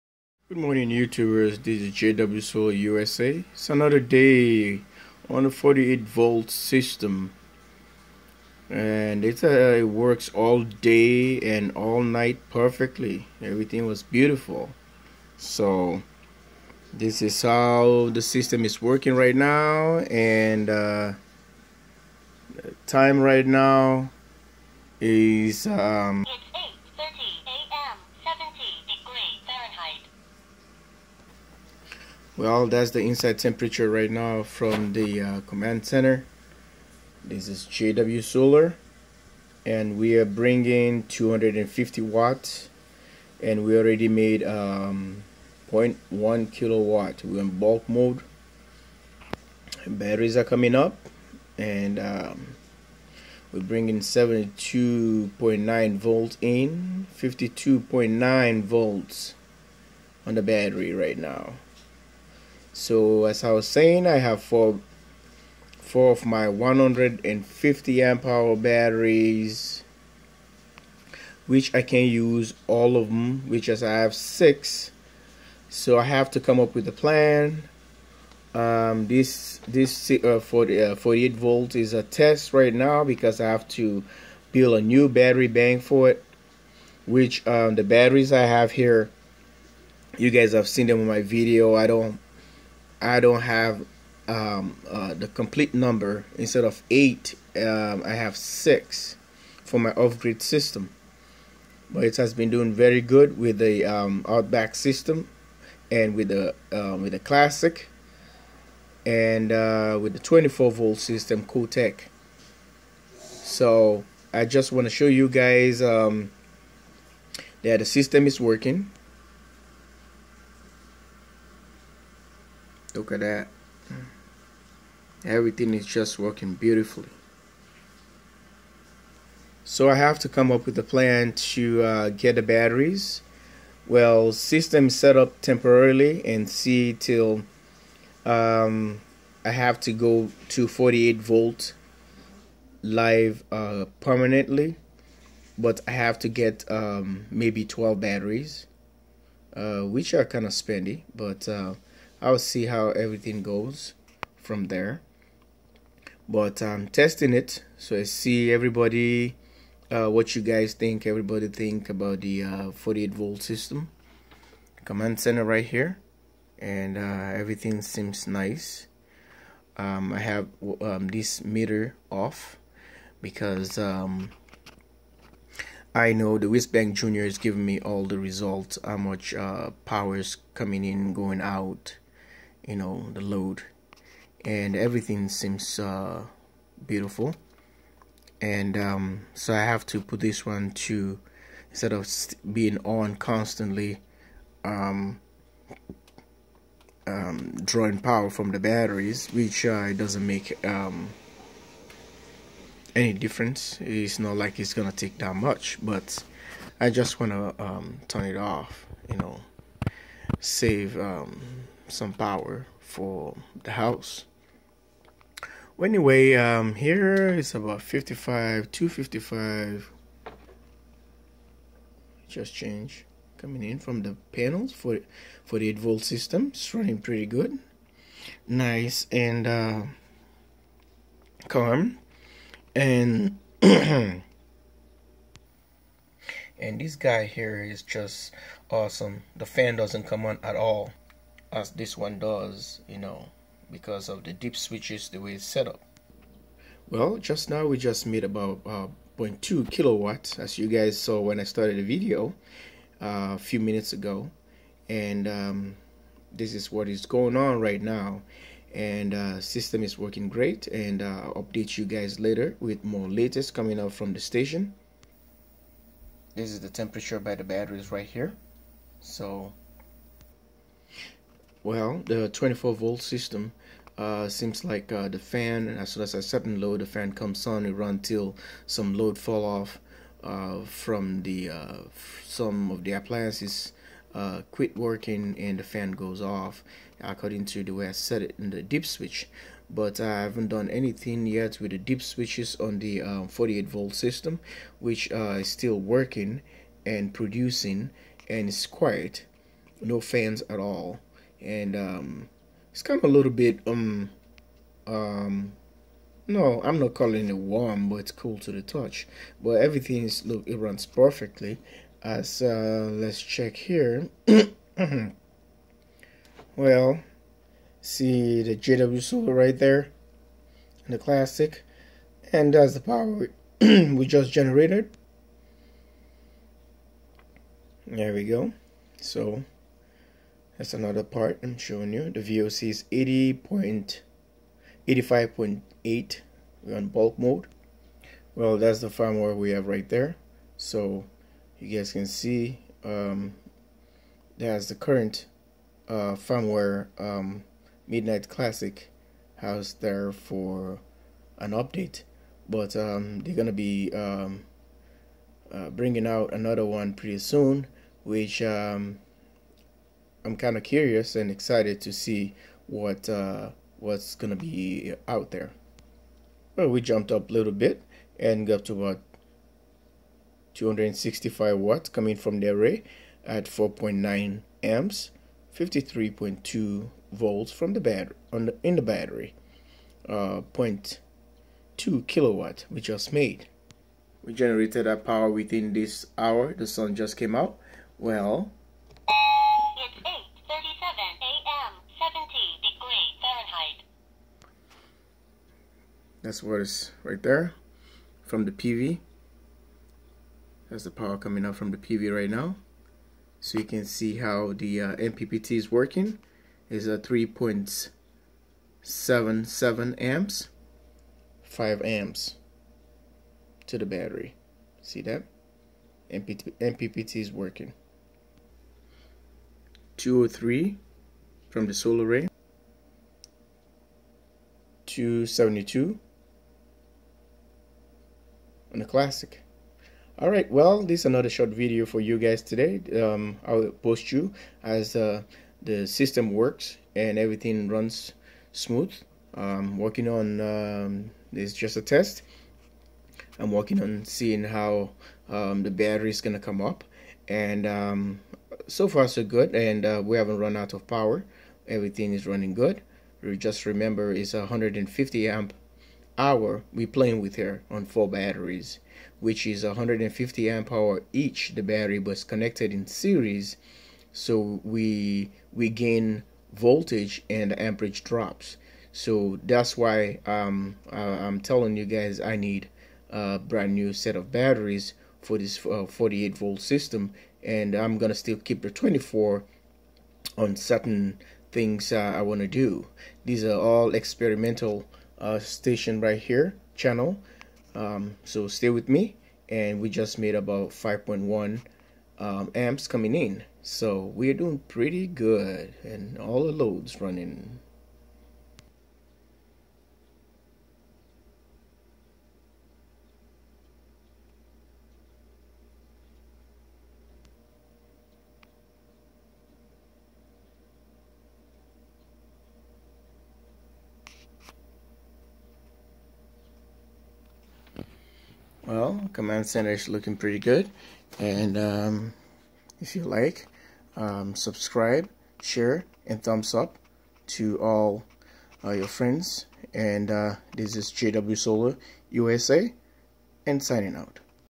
DIY 48V OFF-GRID SOLAR AND BATTERY BACKUP SYSTEM TEST #2
48V Solar OffGrid System which convert light into electricity.
Solar energy is the most efficient and conventional source of energy delivering Eco-friendly energy to the people.
It also fulfills the needs of the people and lower their electricity bill.

PLEASE STAY TUNE FOR MORE VIDEOS AND EDUCATIVE INFORMATION ON THIS CHANNEL.

+jwsolarusa